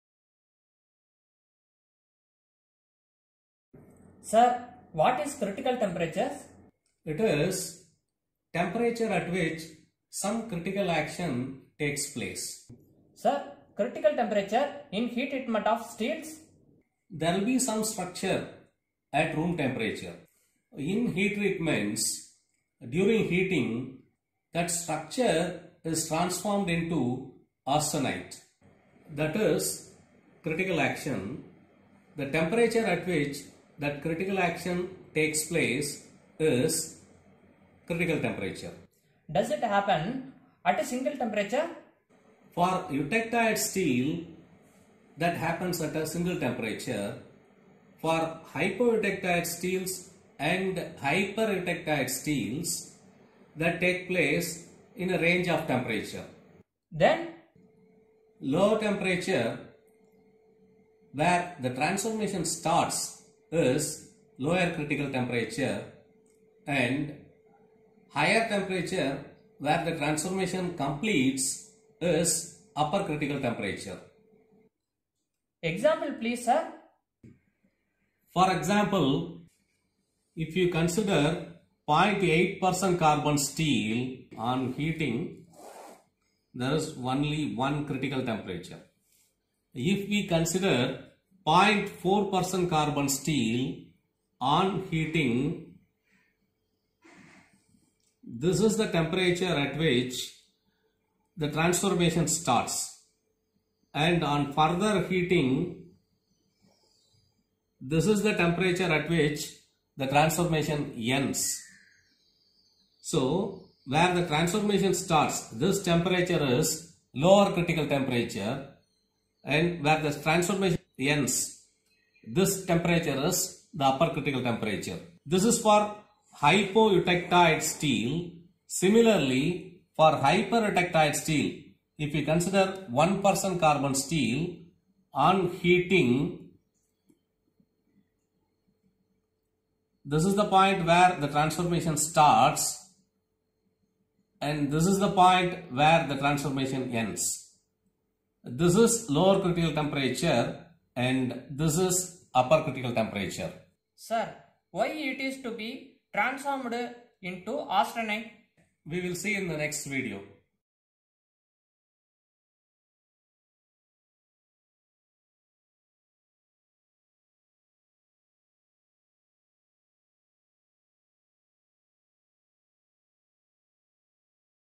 Critical Temperature(s) in Heat Treatments of Steel
What is Critical Temperature in Heat Treatments of Steel? Just an introduction.